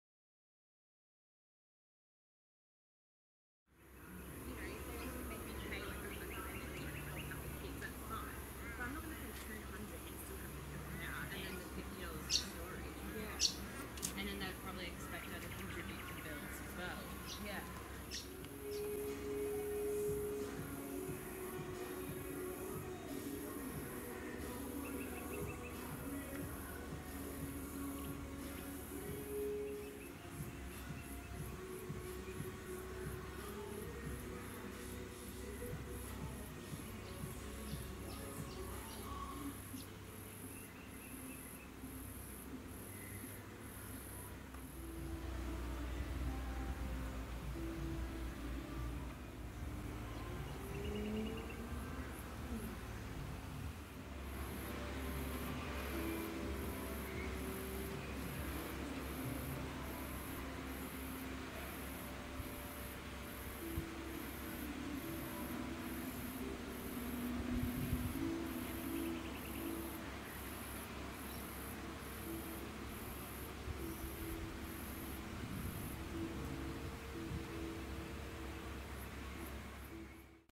Varied Eggfly 7
Varied Eggfly butterfly. Scientific name: Hypolimnas bolina. A female, filmed on the Sunshine Coast, Queensland, in April 2020